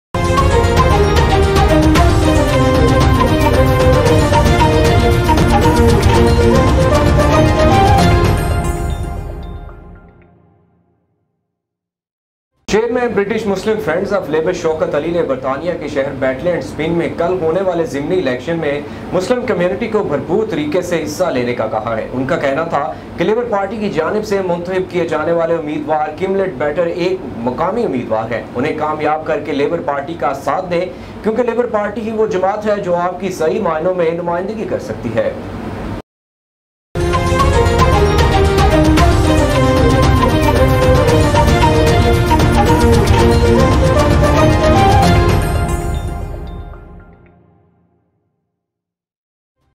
چیئرمین برٹش مسلم فرینڈز آف لیبر شوکت علی :کل ہونے والے ضمنی الیکشن میں مسلم کمیونٹی
چیئرمین برٹش مسلم فرینڈز آف لیبر شوکت علی :کل ہونے والے ضمنی الیکشن میں مسلم کمیونٹی کو بھرپور طریقے سے حصہ لے